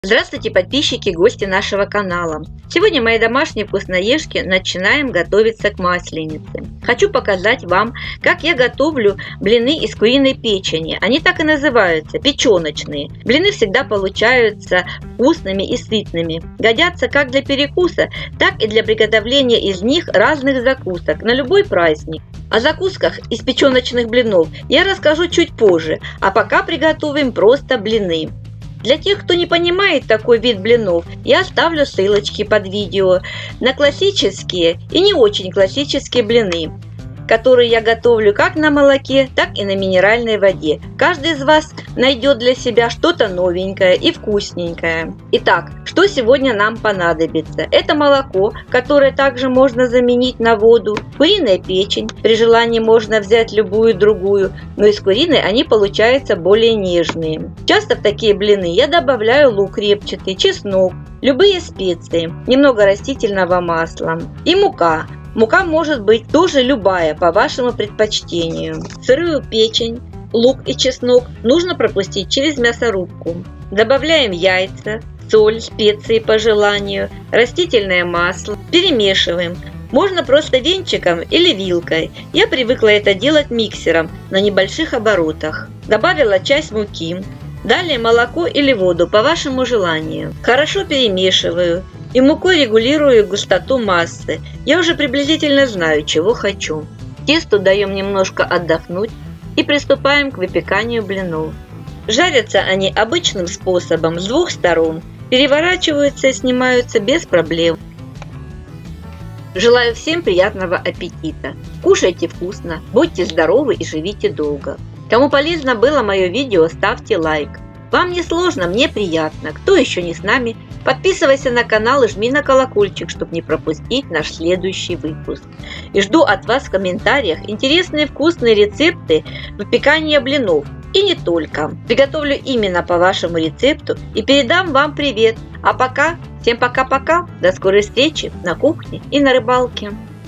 Простыми блинами уже никого не удивишь! Обалденные печеночные блинчики на молоке. Маслиница.
Блинами уже никого не удивишь! Обалденные печеночные блинчики, из которых потом можно приготовить всевозможные закуски , как на праздничный стол, так и для каждодневного перекуса.
ПРОДУКТЫ:
- печень куриная - 600-700гр.
- молоко - 900гр
- растительное масло - 4-5ст.л.
- яйца - 2шт
- соль -1ч.л.
- перец - по вкусу
- мука - 8ст.л.
- лук - 1головка
- чеснок - по желанию